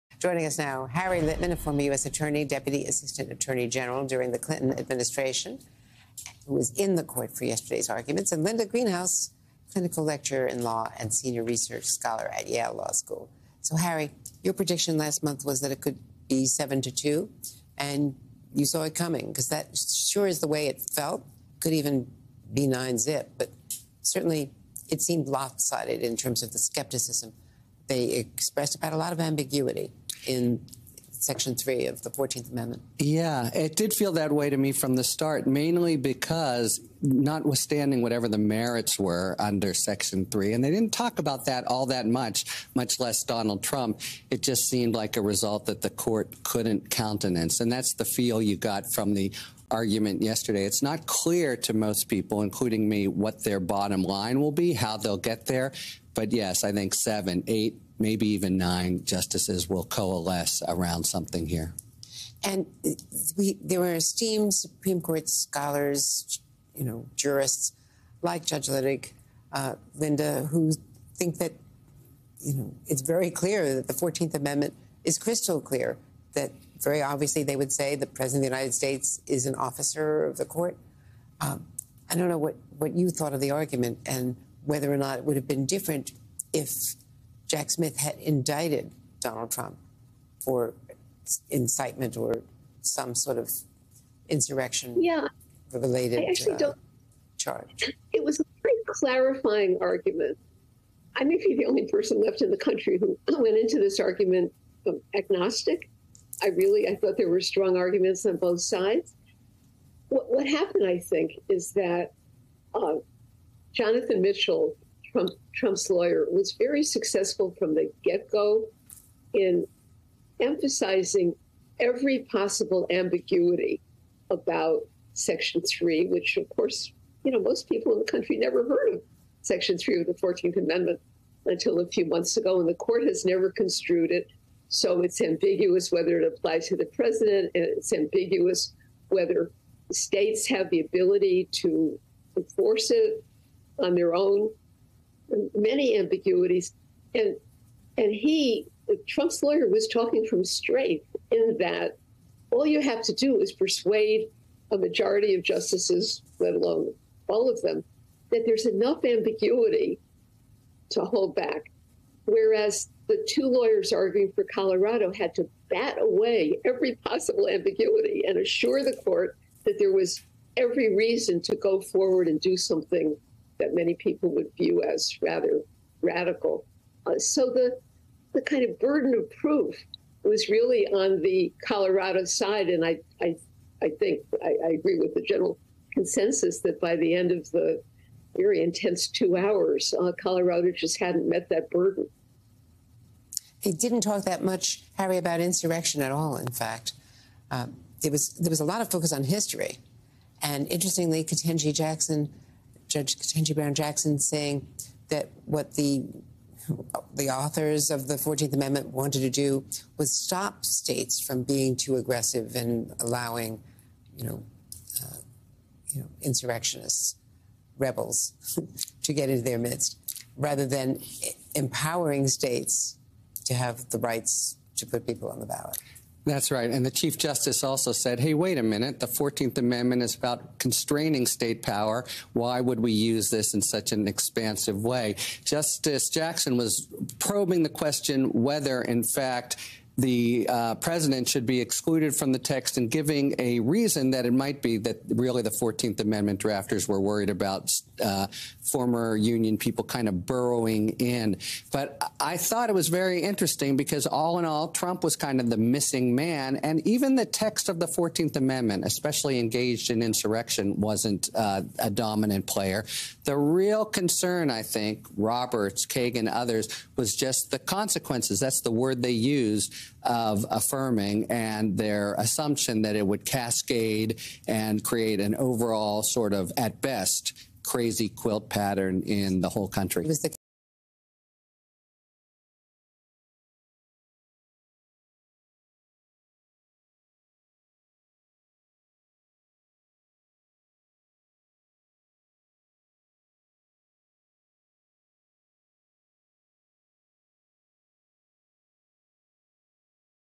Greenhouse: ‘Colorado just hadn’t met’ the ‘burden of proof’ to sway SCOTUS to bar Trump from ballot
The case made by Colorado lawyers to bar former President Donald Trump for violating Section 3 of the 14th Amendment was met with serious skepticism by the justices of the Supreme Court during oral arguments. Harry Litman and Linda Greenhouse join Andrea Mitchell to weigh in on how the court could rule. “Jonathan Mitchell, Trump's lawyer, was very successful from the get-go in emphasizing every possible ambiguity about Section 3,” Greenhouse says. “Whereas the two lawyers arguing for Colorado had to bat away every possible ambiguity and assure the court that there was every reason to go forward and do something that many people would view as rather radical.”